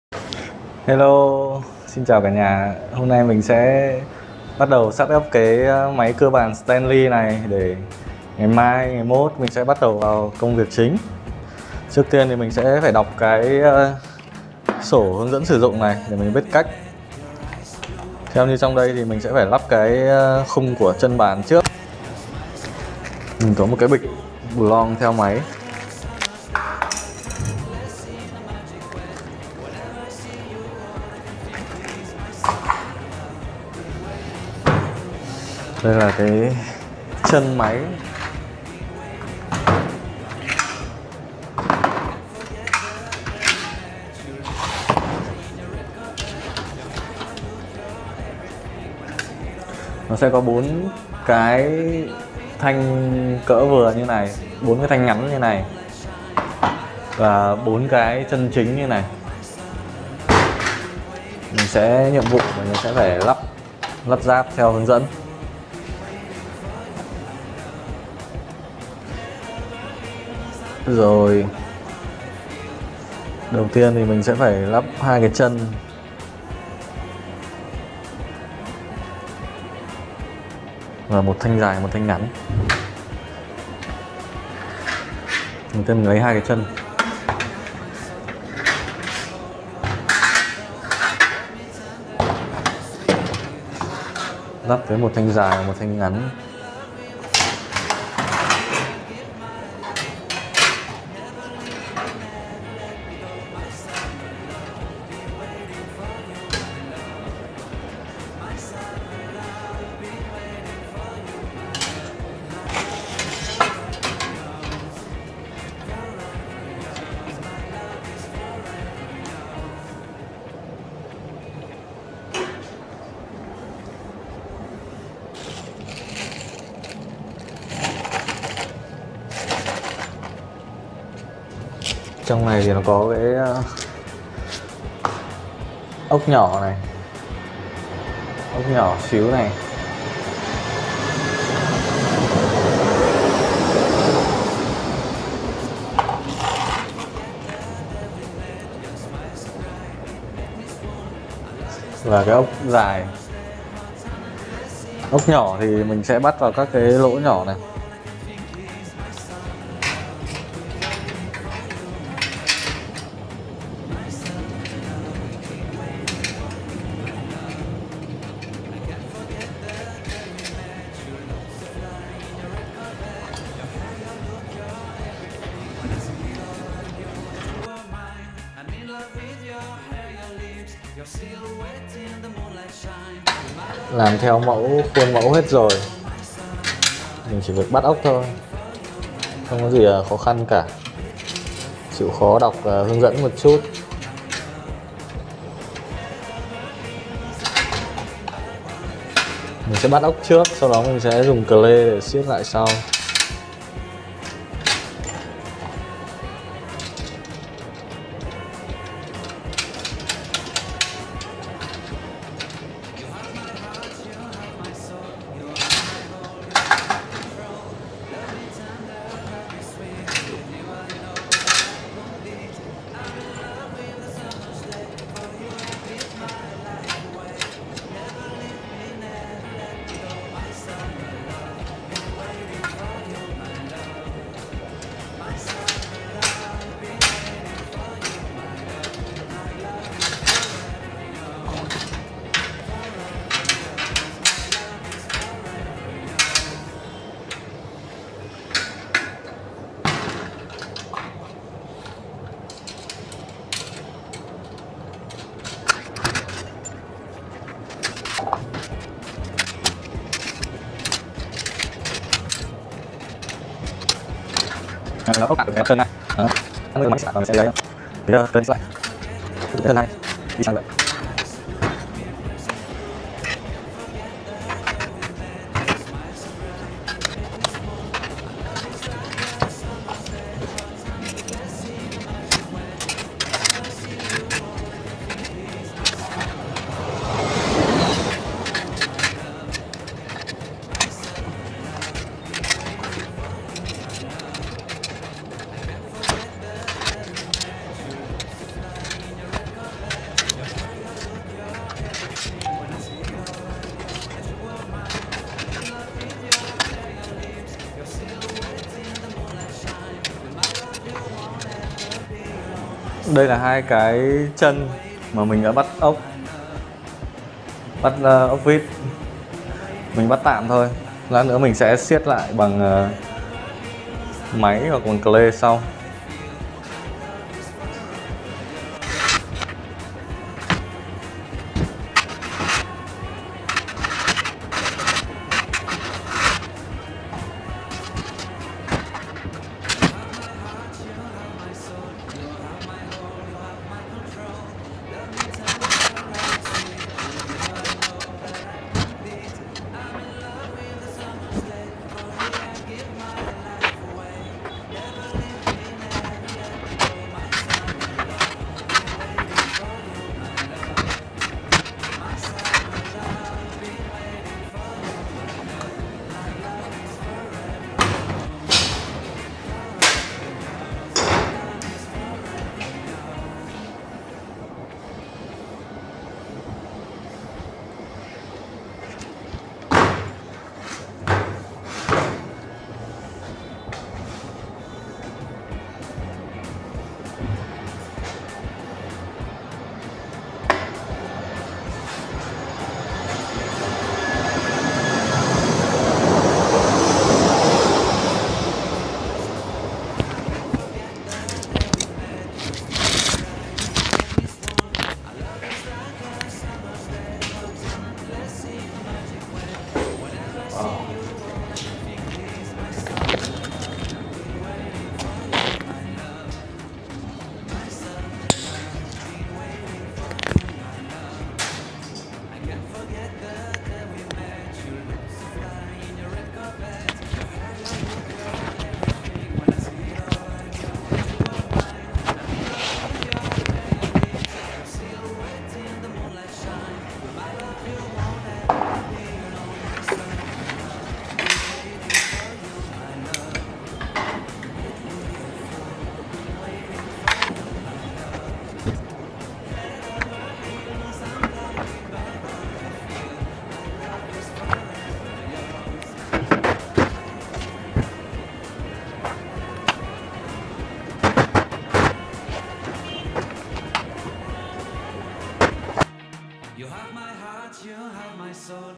Tự làm quầy cafe specialty - trang trí quán cafe -p2-eps2- ráp chân máy cưa bàn stanley
Mấy hôm nay mình lubu một số công việc khác nên chỉ tranh thủ ráp được cái chân máy cưa bàn stanley.
Máy vẫn chưa chạy hihi
Ráng thu xếp để triển tiếp thôi